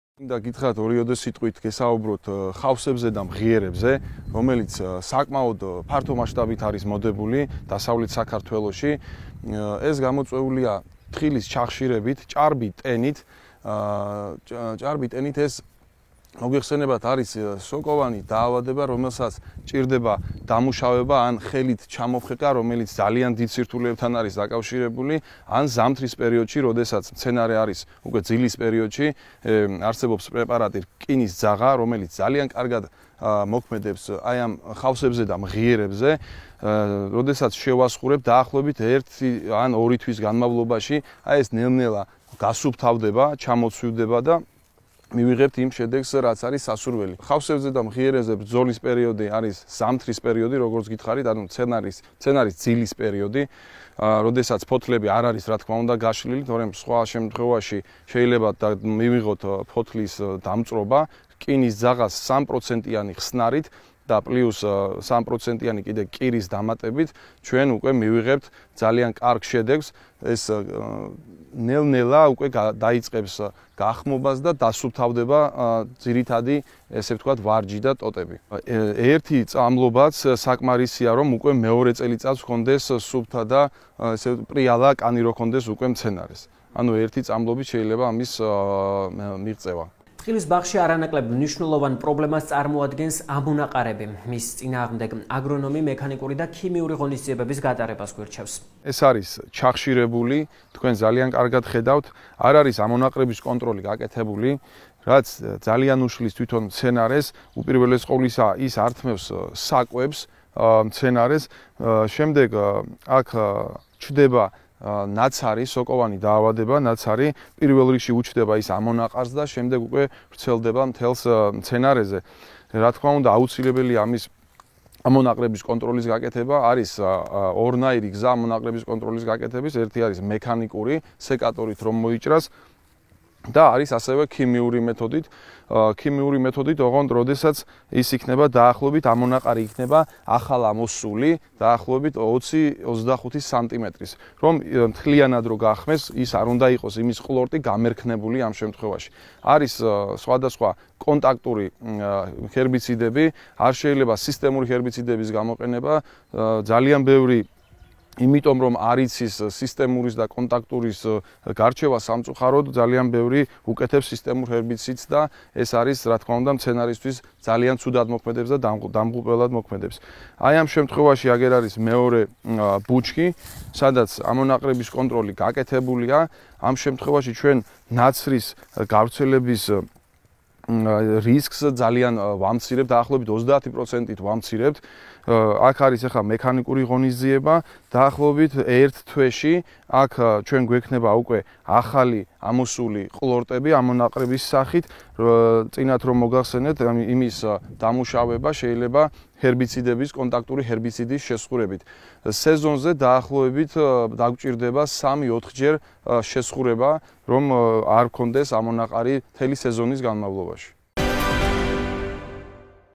ხავსები და მღიერები თხილის მეურნეობაში
როგორ ვებრძოლოთ ხავსებს თხილის ბაღში და რა უნდ ავიცოდეთ მღიერებთან ბრძოლისას? აგრო რჩევები "აგროარემოtv"